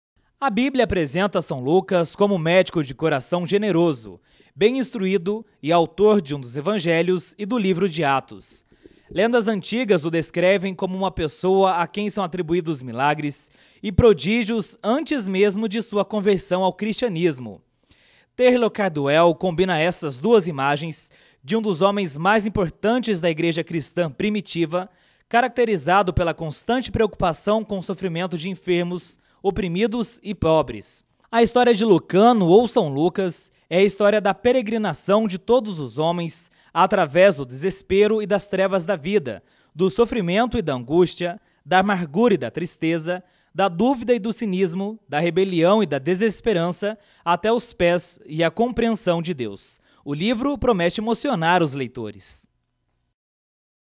História de São Lucas é a nossa 'Dica de Leitura' de hoje
Você já conhece a história de São Lucas? Ainda não? A nossa Dica de Leitura de hoje, 'Médico de Homens e de Almas', conta a história deste santo da Igreja Católica. Acompanhe.